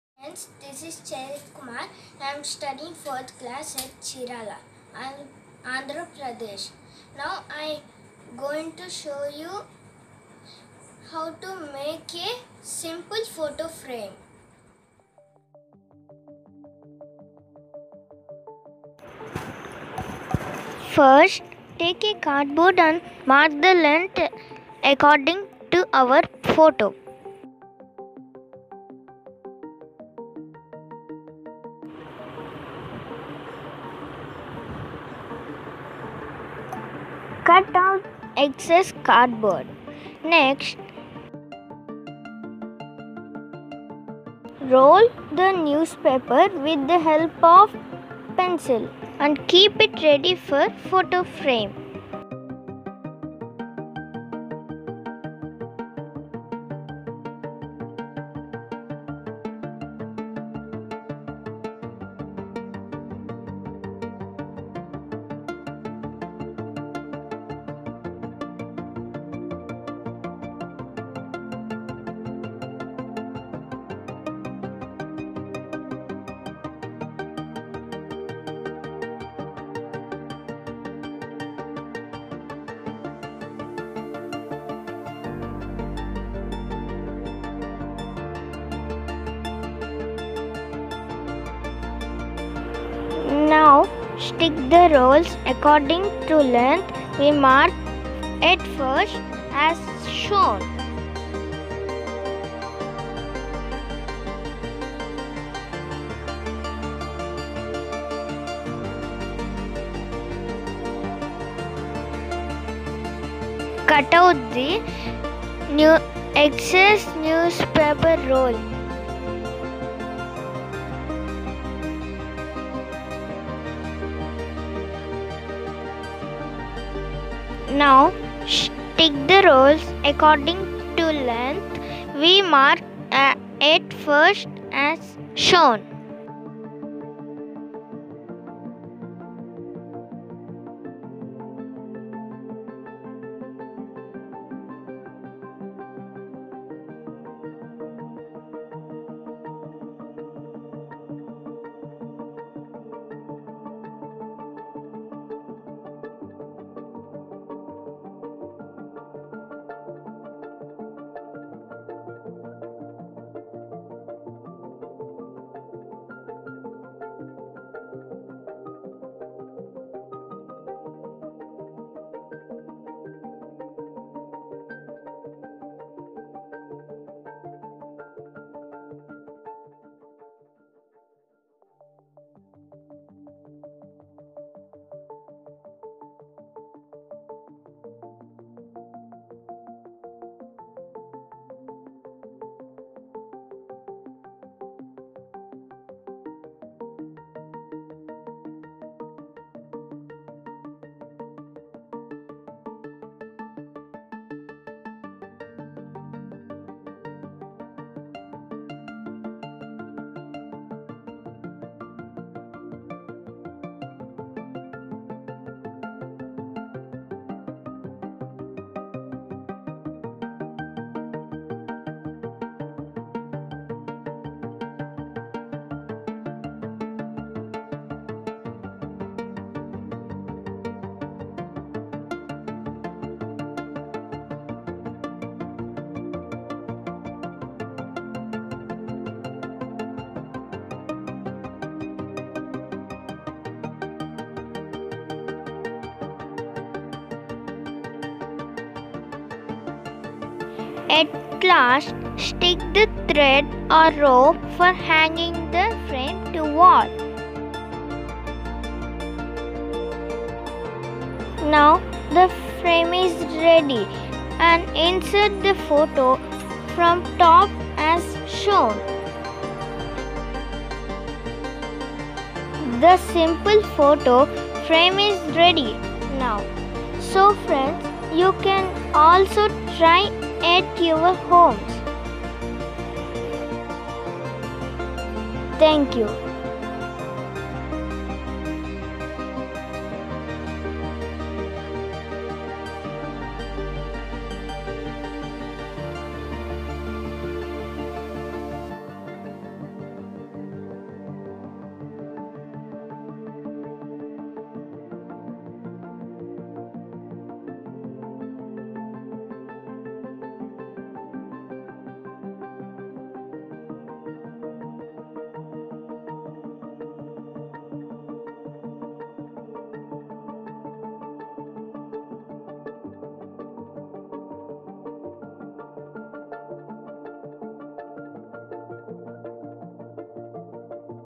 How to make an easy photo frame at home | telugu | photo frame easy making at home gayathris diy
hi friends, in this video I'm going to show you how to make easy photo frame at home easily with newspaper and cardboard..